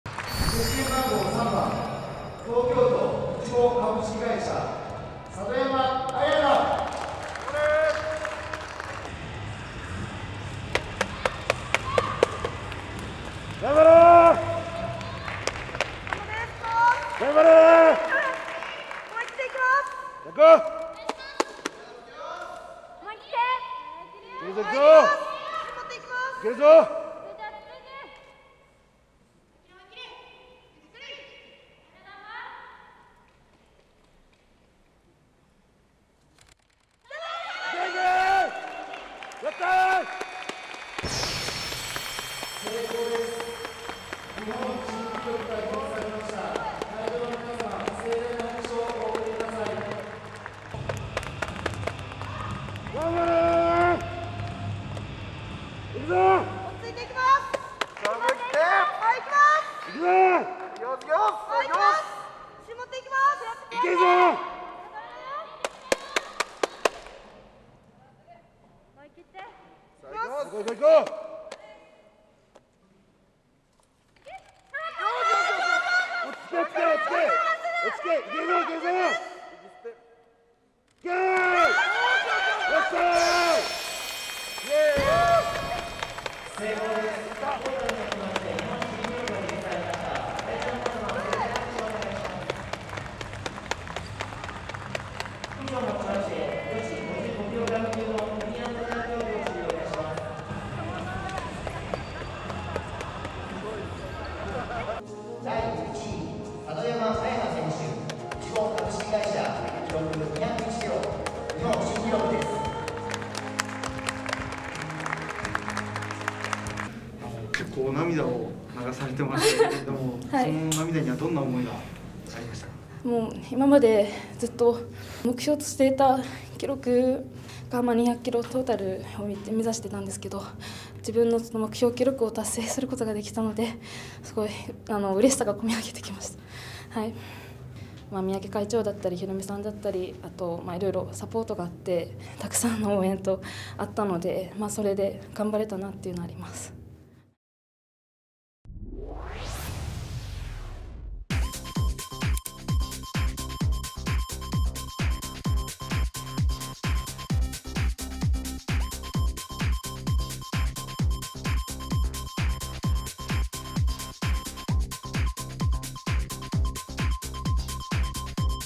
「感謝と涙」の日本新記録！宮古島出身、三宅親子の愛弟子が快挙！【ウエイトリフティング】
重量挙げの全日本選手権（21日、山梨市民総合体育館）が行われ、女子55キロ級は宮古島出身の佐渡山彩奈（28、いちご）がスナッチで日本新記録の90キロ、クリーン＆ジャークでは111キロの大会新記録をマーク。トータル201キロの日本新で、2連覇を果たした。
所属するいちごウエイトリフティング部では、レジェンド・三宅義行さん（メキシコシティ五輪銅）、宏実さん（ロンドン五輪銀、リオ五輪銅）に師事。佐渡山は「今まで目標にしていた記録を達成して、嬉しさがこみ上げてきた」と涙を流した。